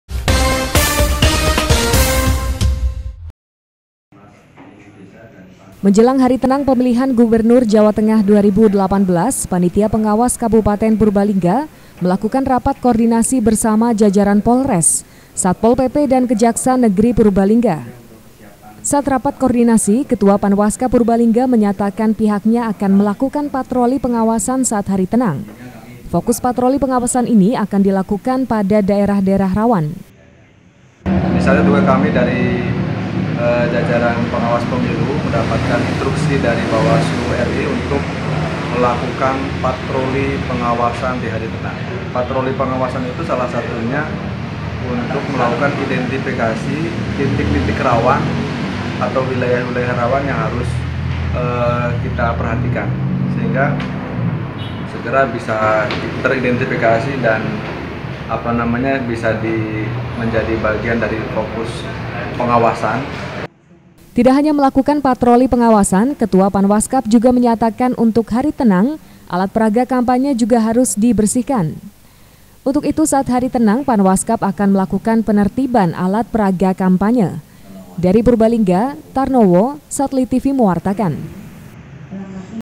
HARI TENANG PANWASKAB PURBALINGGA AKAN LAKUKAN PATROLI PENGAWASAN
WARTA SORE. Menjelang hari tenang Pilgub Jawa Tengah 2018, Panwaskab Purbalingga lakukan rapat koordinasi mengenai patroli pengawasan selama hari tenang.